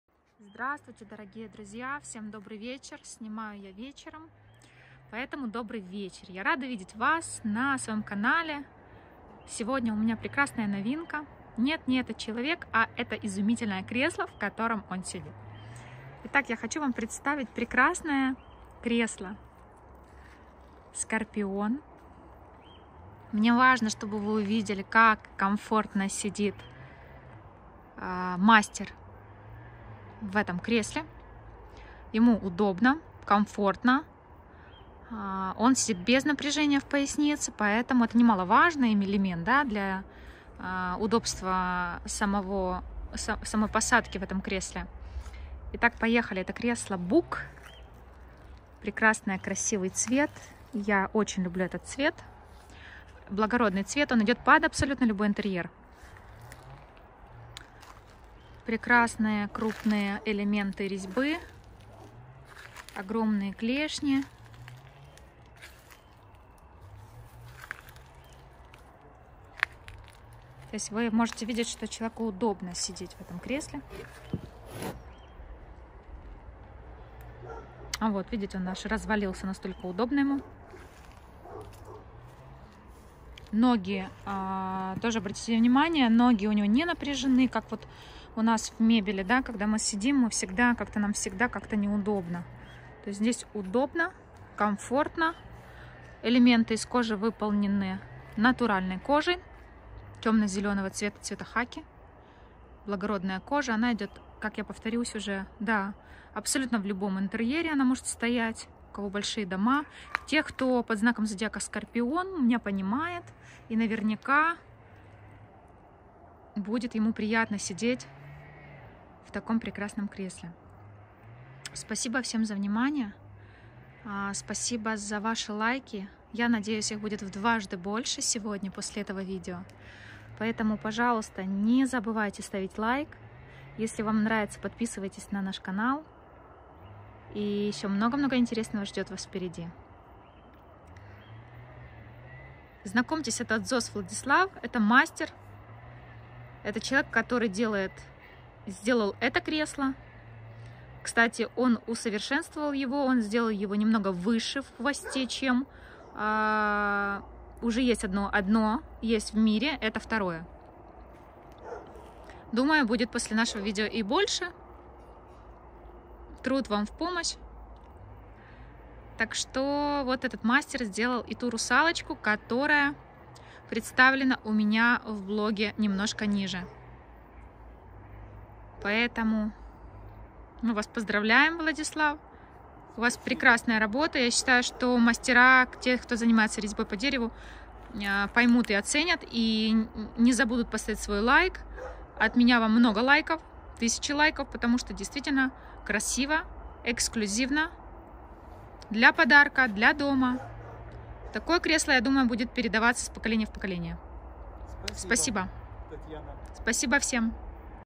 Wood Carving Scorpion Armchair Woodworking Art Top Amazing
Scorpion carved wooden chair, author Dzoz Vladyslav - Резное деревянное кресло Скорпион, автор Дзоз Владислав.